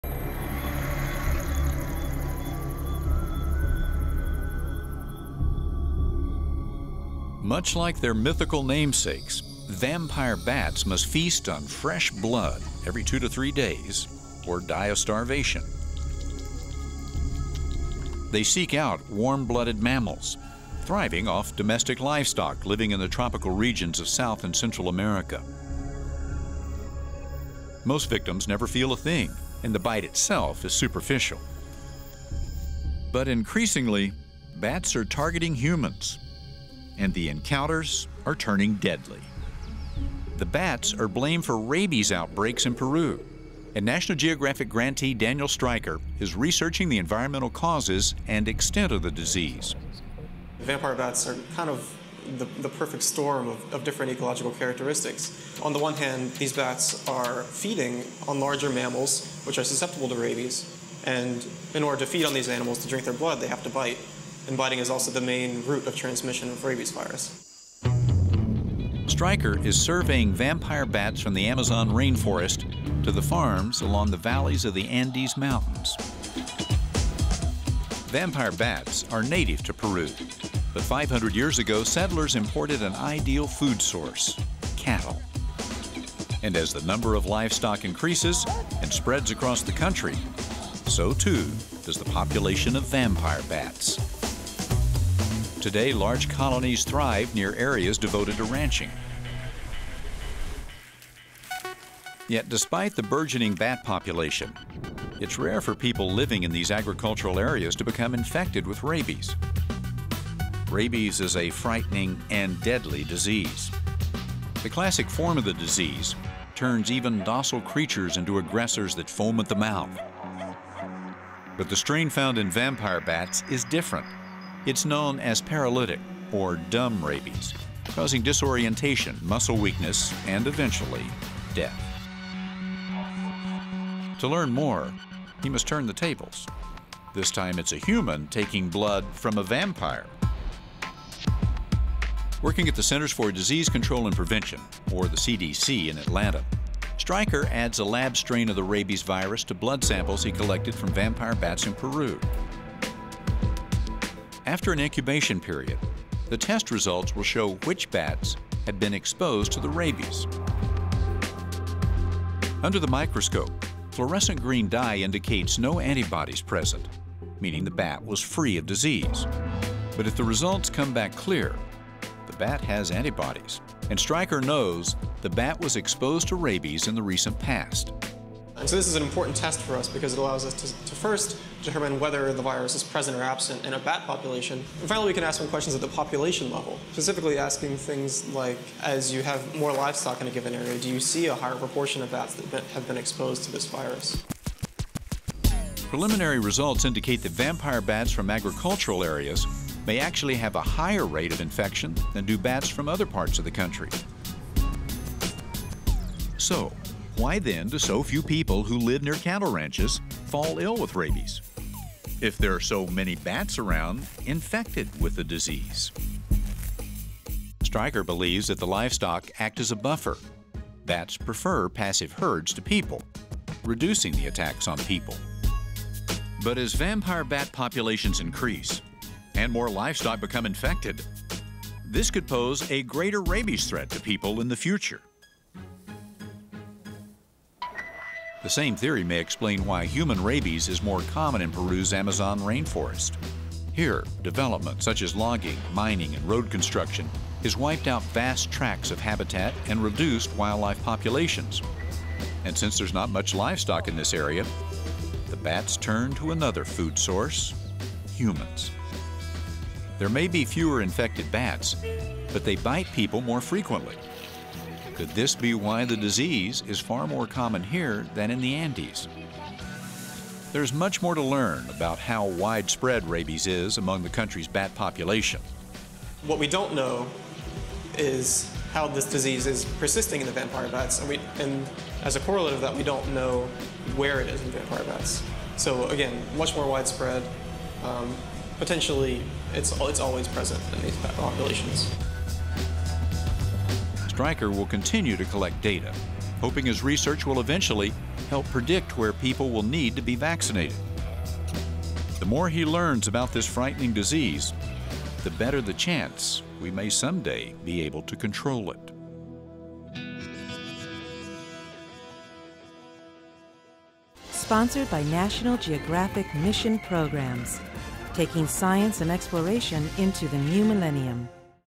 Vampire Bats Biting People | National Geographic
Vampire bats in Peru are increasingly biting people, and a National Geographic researcher is trying to find ways to stem the resulting spread of deadly rabies.